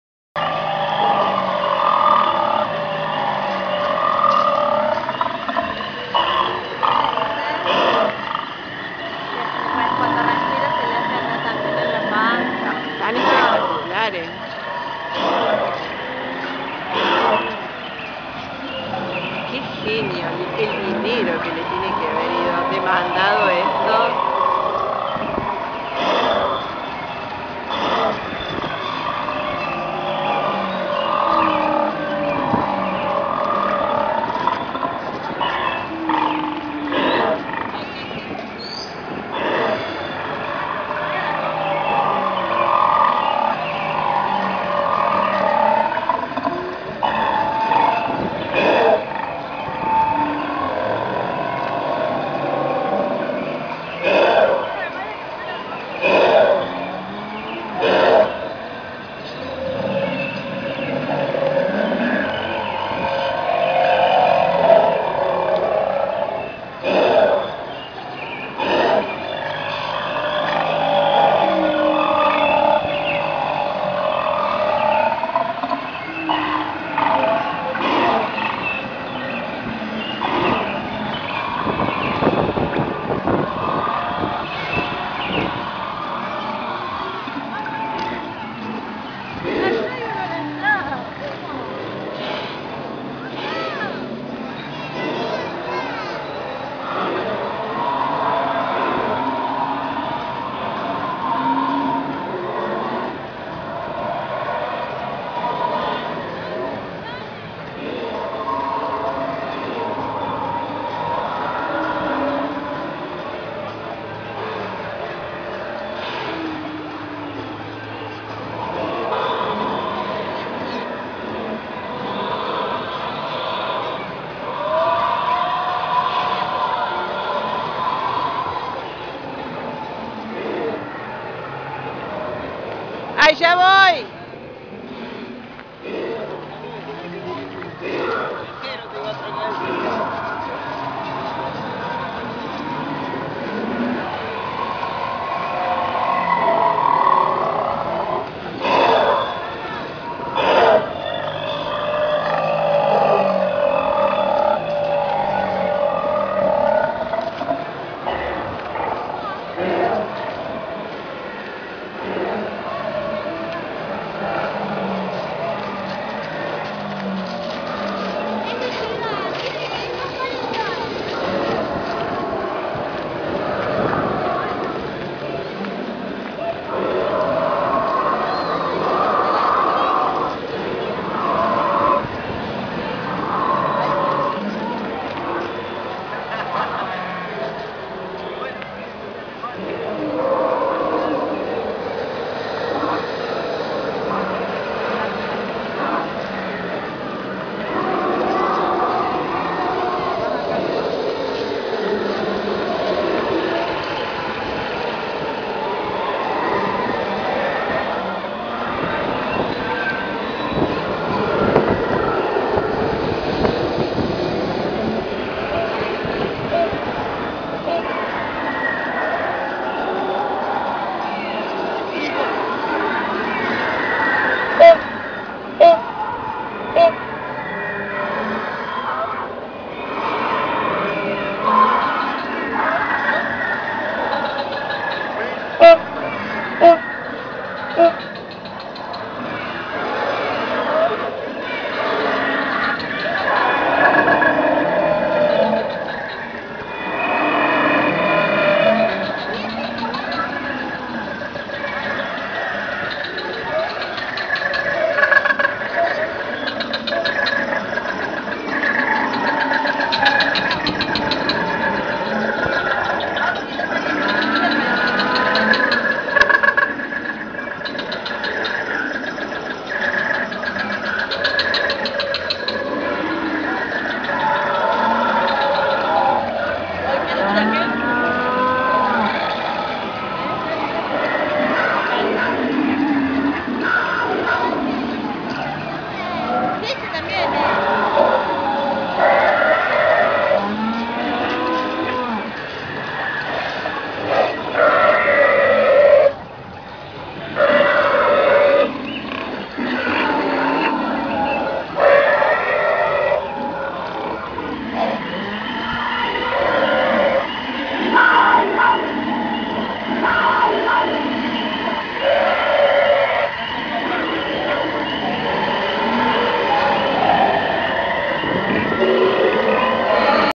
Tecnópolis 2012 - Parque de Dinosaurios.AVI
Pequeño vídeo tomado en Tecnópolis - Buenos Aires - 2012 - Parque de Dinosaurios.